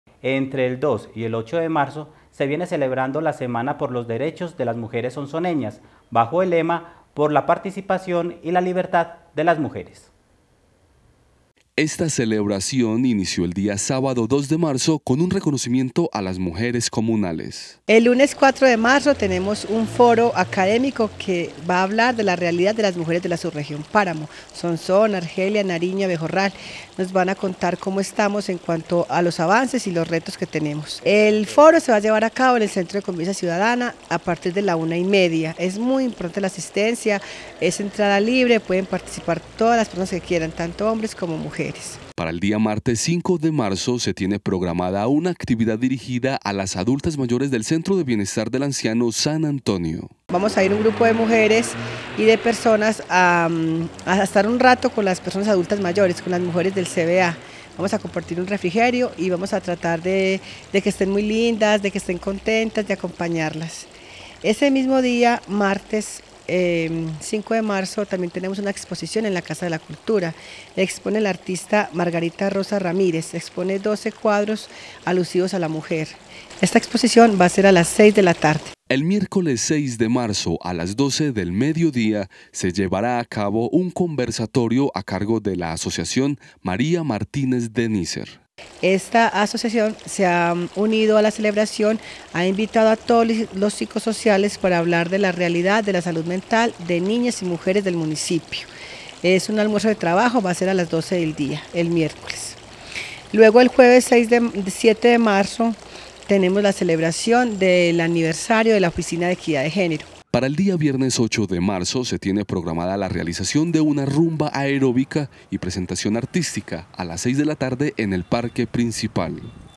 PROGRAMACIÓN SEMANA DE LA MUJER SONSONEÑA
Entre el 2 y el 8 de marzo se viene celebrando la Semana por los Derechos de las Mujeres Sonsoneñas, bajo el lema “Por la participación y la libertad de las mujeres”.